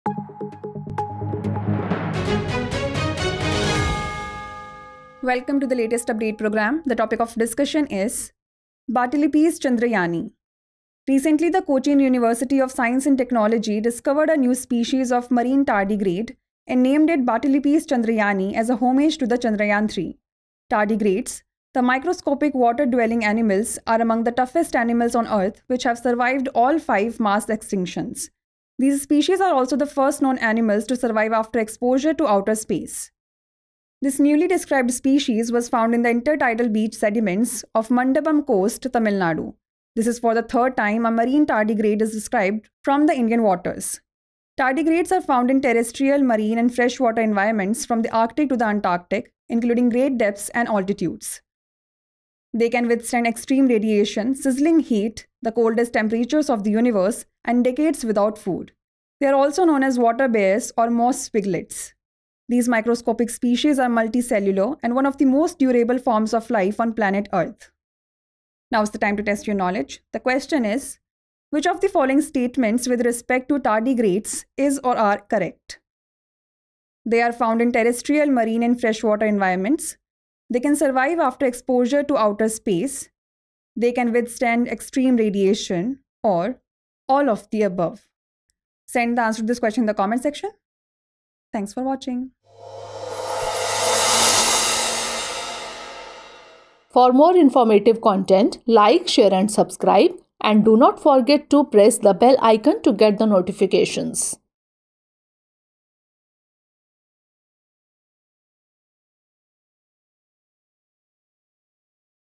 Batillipes Chandrayaani | Tardigrades | Chandrayaan 3 | Latest update | Drishti IAS English
Recently Cochin University of Science and Technology have discovered a new species of marine tardigrade and named it as Batillipes Chandrayaani, as a homage to the Chandrayaan-3. Tardigrades, the microscopic water-dwelling animals, are among the toughest animals on earth which have survived all five mass extinctions.